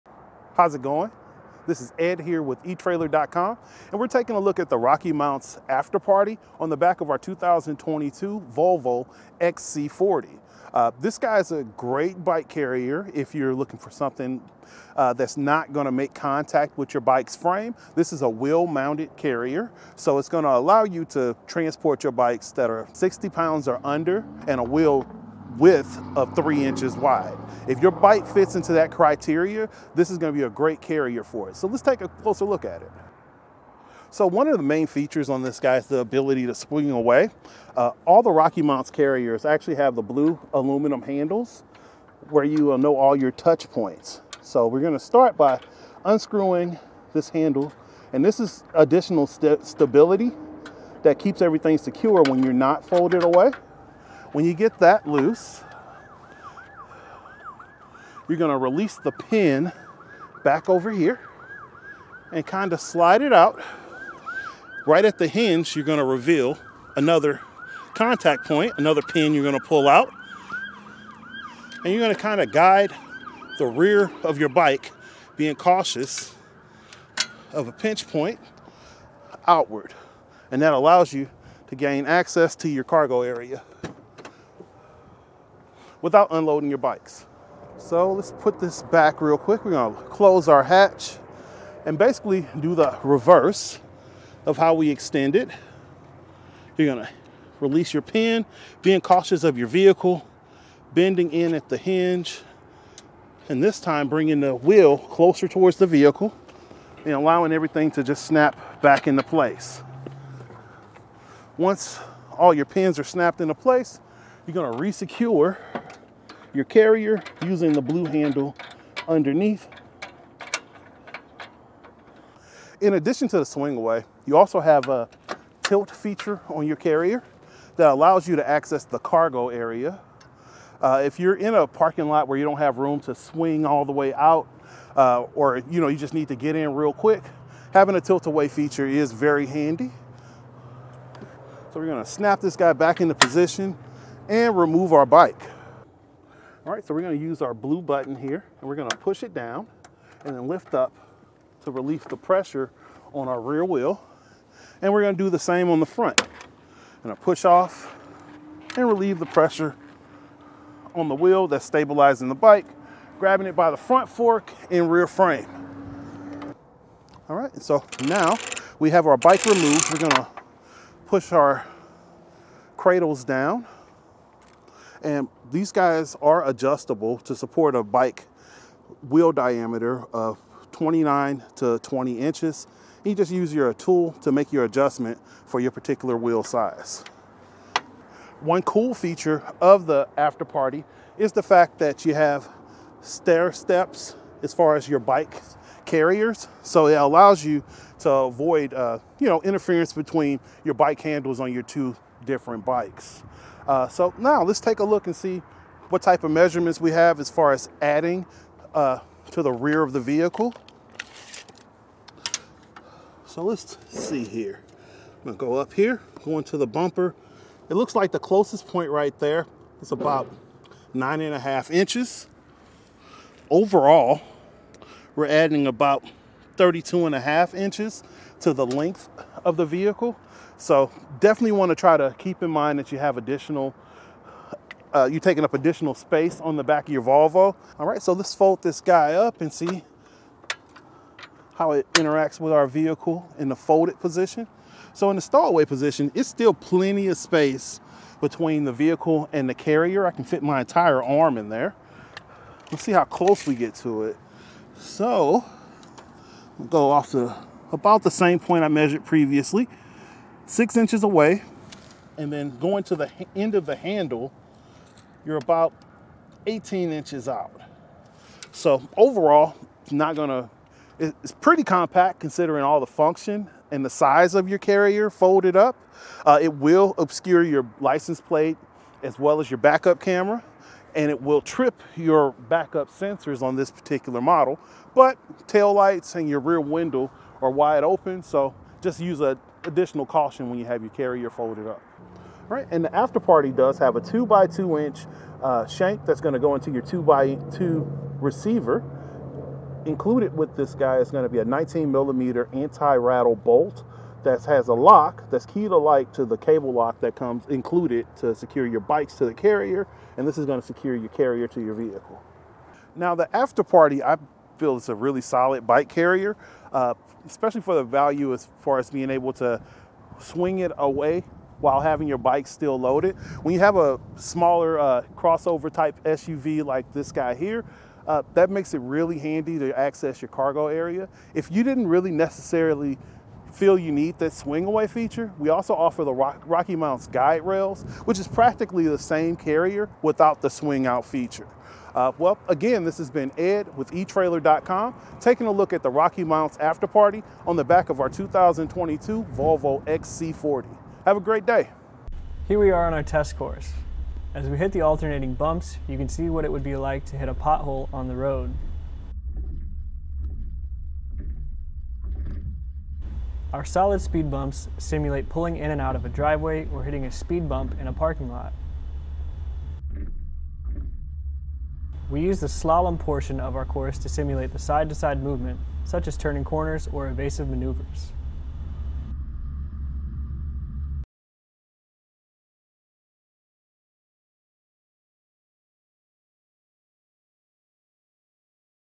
How to Install: RockyMounts AfterParty Swing Away Bike Rack for 2 Bikes on your 2022 Volvo XC40
How's it goin' This is Ed here with eTrailer.com, and we're takin' a look at the RockyMounts AfterParty on the back of our 2022 Volvo XC40.  This guy's a great bike carrier, if you're looking for something that's not gonna make contact with your bike's frame.  This is a wheel-mounted carrier, so it's gonna allow you to transport your bikes that are 60 pounds or under and a wheel width of three inches wide.  If your bike fits into that criteria, this is gonna be a great carrier for it.  So let's take a closer look at it.  So one of the main features on this guy is the ability to swing away.

All the RockyMounts carriers actually have the blue aluminum handles, where you will know all your touchpoints.  So we're gonna start by unscrewing this handle, and this is additional stability that keeps everything secure when you're not folded away.  When you get that loose, you're gonna release the pin back over here, and kind of slide it out.  Right at the hinge you're gonna reveal another contact point, another pin you're gonna pull out, and you're gonna kinda guide the rear of your bike, being cautious of a pinch point, outward, and that allows you to gain access to your cargo area without unloadin' your bikes.  So let's put this back real quick.

We're gonna close our hatch, and basically do the reverse of how we extended.  You're gonna release your pin, being cautious of your vehicle, bending in at the hinge, and this time bringin' the wheel closer towards the vehicle, and allowing everythin' to just snap back into place.  Once all your pins are snapped into place, you're gonna re-secure your carrier using the blue handle underneath.  In addition to the swing away, you also have a tilt feature on your carrier that allows you to access the cargo area.  If you're in a parking lot where you don't have room to swing all the way out, or, you know, you just need to get in real quick, having a tilt away feature is very handy.

So we're gonna snap this guy back into position and remove our bike.  All right, so we're gonna use our blue button here, and we're gonna push it down and then lift up to release the pressure on our rear wheel.  And we're gonna do the same on the front.  Gonna push off and relieve the pressure on the wheel that's stabilizing the bike, grabbin' it by the front fork and rear frame.  All right, so now we have our bike removed.

We're gonna push our cradles down, and these guys are adjustable to support a bike wheel diameter of 29 to 20 inches.  You just use your tool to make your adjustment for your particular wheel size.  One cool feature of the AfterParty is the fact that you have stair steps as far as your bike carriers.  So it allows you to avoid, you know, interference between your bike handles on your two different bikes.  So now let's take a look and see what type of measurements we have as far as adding to the rear of the vehicle.  So let's see here.  I'm gonna go up here, go into the bumper.  It looks like the closest point right there is about nine and a half inches.  Overall, we're adding about 32 and a half inches to the length of the vehicle.  So, definitely wanna try to keep in mind that you have additional, you're taking up additional space on the back of your Volvo.  All right, so let's fold this guy up, and see how it interacts with our vehicle in the folded position.  So in the stow-away position, it's still plenty of space between the vehicle and the carrier.  I can fit my entire arm in there.  Let's see how close we get to it.  So, we'll go off to about the same point I measured previously.  Six inches away.  And then going to the end of the handle, you're about 18 inches out.  So overall it's not gonna.  It's pretty compact considering all the function and the size of your carrier folded up.  It will obscure your license plate as well as your backup camera, and it will trip your backup sensors on this particular model, but taillights and your rear window are wide opens.  So just use additional caution when you have you carrier folded up.  All right, and the AfterParty does have a two-by-two-inch shank that's gonna go into your two-by-two receiver.  Included with this guy is gonna be a 19-millimeter anti-rattle bolt that has a lock that's key (indistinct) to the cable lock that comes included to secure your bikes to the carrier, and this is gonna secure your carrier to your vehicle.  Now